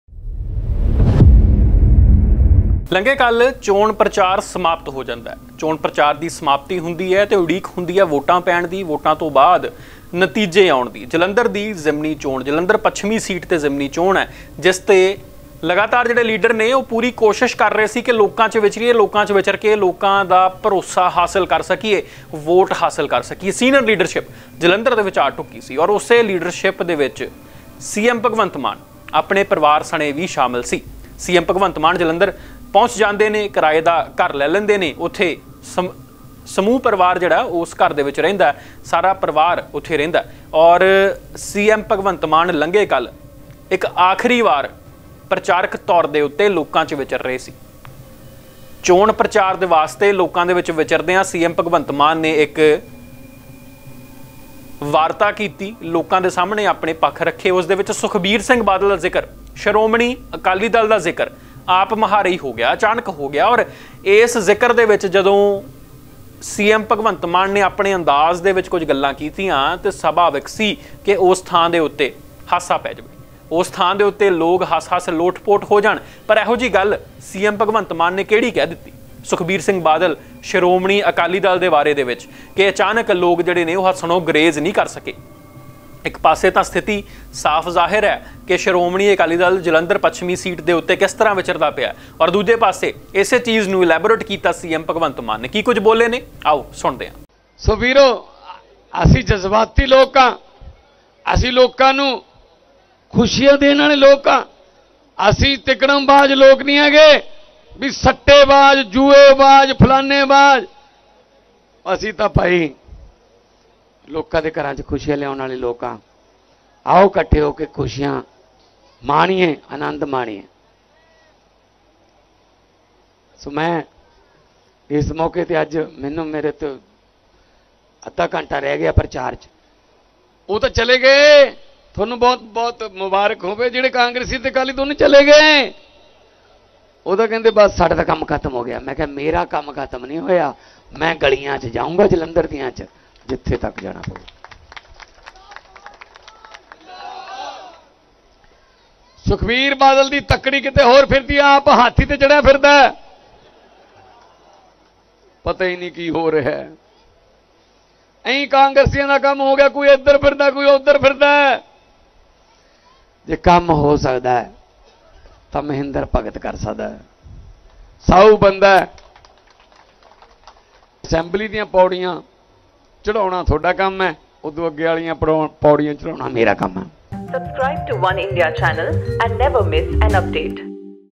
"ਸੁਖਬੀਰ ਬਾਦਲ ਹਾਥੀ 'ਤੇ ਚੜ੍ਹਿਆ ਫਿਰਦਾ...!" CM ਮਾਨ ਨੇ ਨਿਹੰਗ ਦੇ ਬਾਟੇ ਵਾਂਗ ਮਾਂਜ'ਤਾ ਬਾਦਲ
"ਸੁਖਬੀਰ ਬਾਦਲ ਹਾਥੀ 'ਤੇ ਚੜ੍ਹਿਆ ਫਿਰਦਾ...!"
CM ਮਾਨ ਨੇ ਨਿਹੰਗ ਦੇ ਬਾਟੇ ਵਾਂਗ ਮਾਂਜ'ਤਾ ਬਾਦਲ
ਬਾਦਲ ਦੀ ਬੇਇਜ਼ਤੀ 'ਤੇ ਹੱਸ ਹੱਸ ਦੁਹਰੇ ਹੋਏ ਲੋਕ